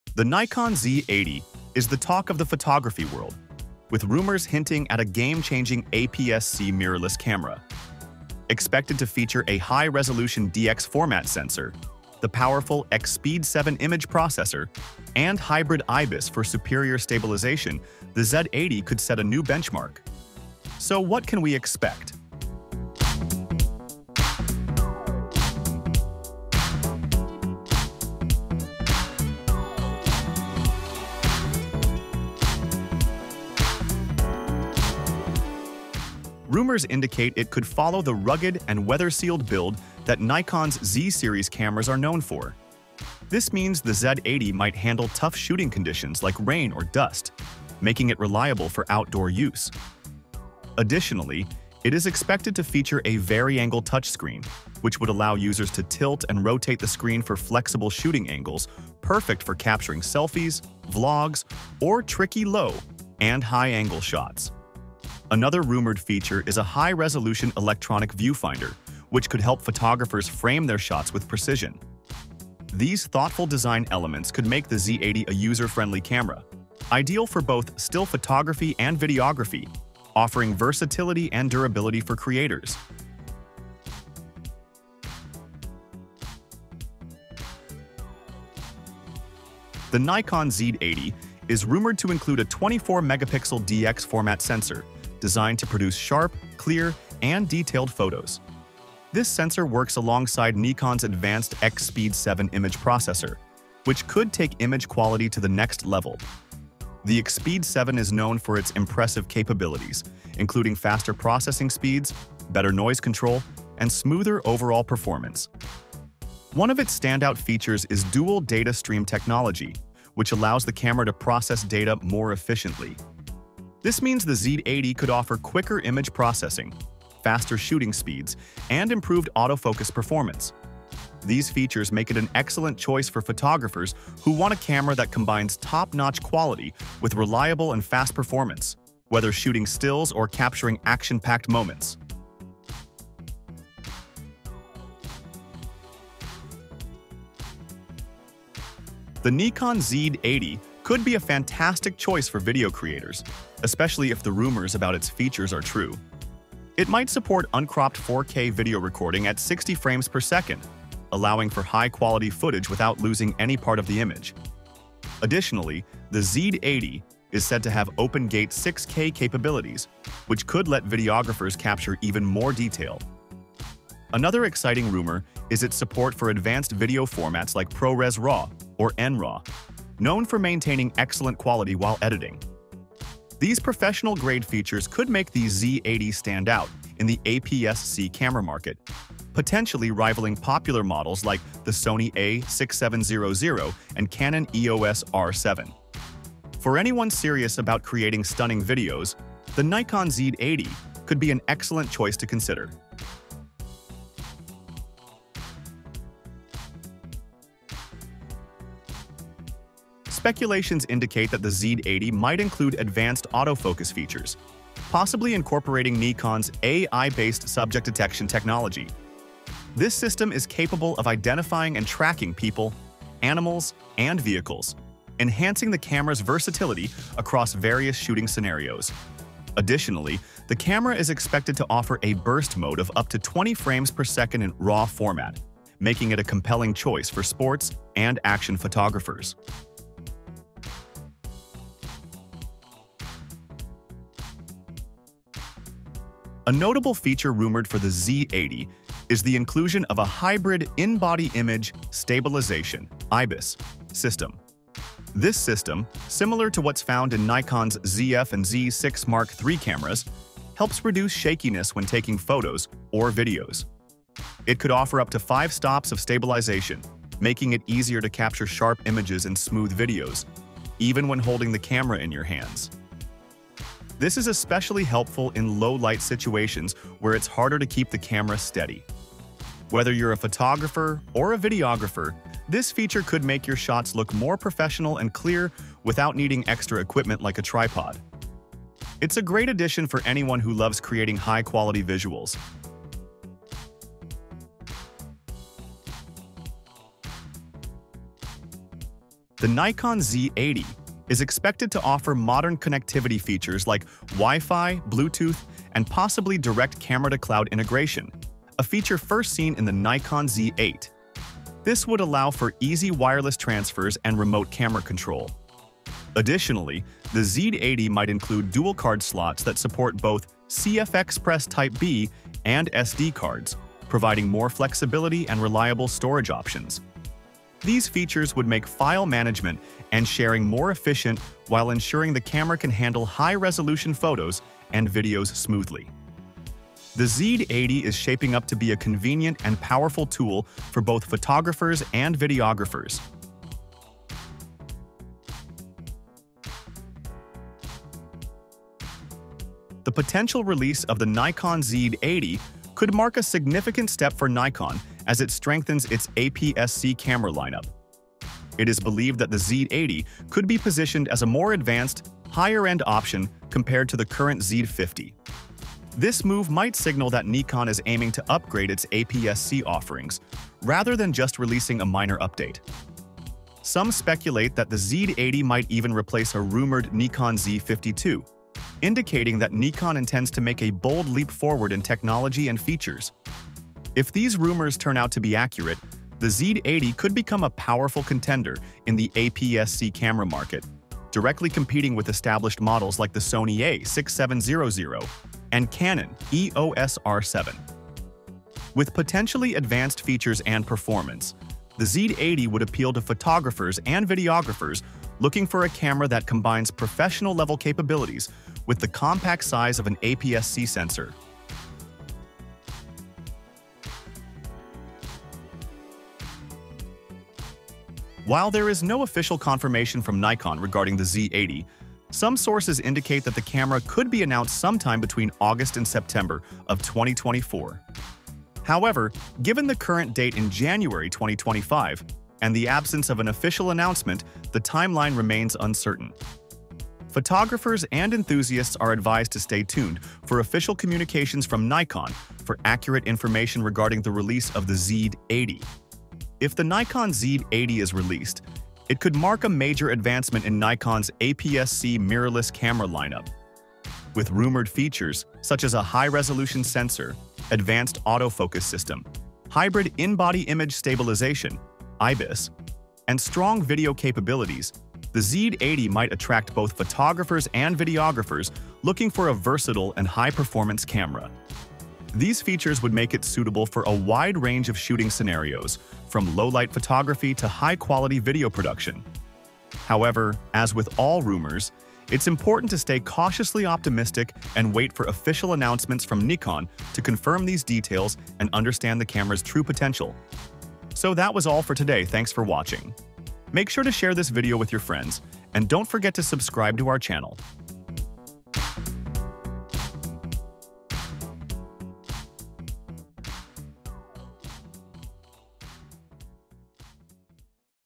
Nikon Z80 - Specs Price Release date 2025!
The upcoming Nikon Z80 is poised to redefine professional mirrorless photography with its anticipated cutting-edge features. Expected to build upon Nikon's innovative heritage, the Nikon Z80 promises to deliver next-generation AI-driven autofocus and enhanced image stabilization. Wildlife and landscape photographers are eagerly awaiting the Nikon Z80's arrival, with its rumored advanced weather sealing, extended battery life, and revolutionary subject recognition system.
 Video Sources:  @nikonusa